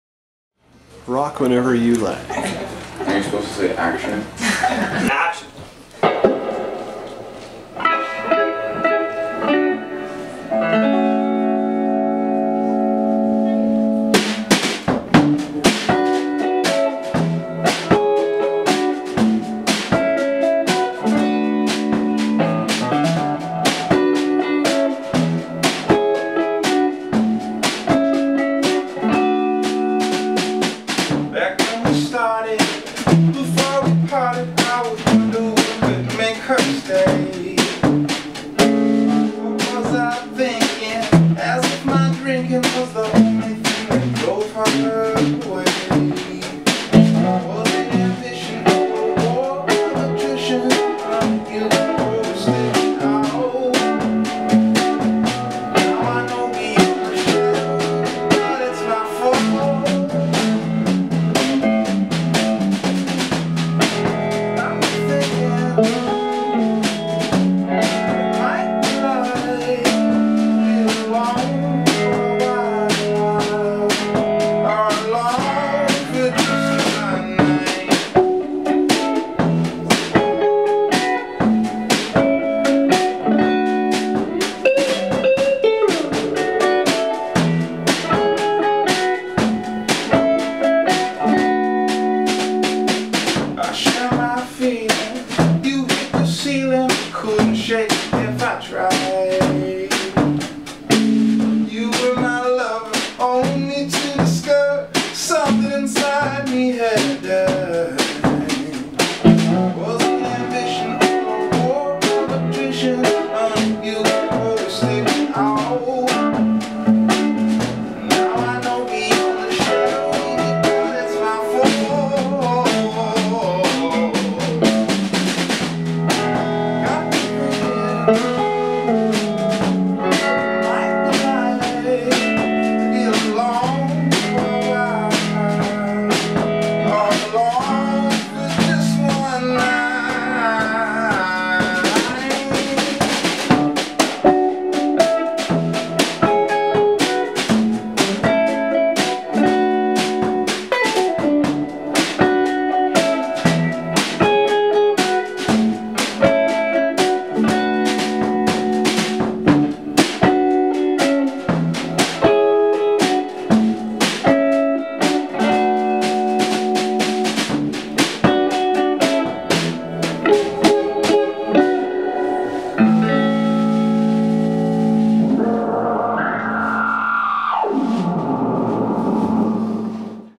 Bahamas @ Canteen.
Toronto's Bahamas (Afie Jurvanen) played a wonderful set at Canteen Art Shop for Bands Undone. Bahamas style of rootsy folk rock is really catchy and a pleasure to listen to.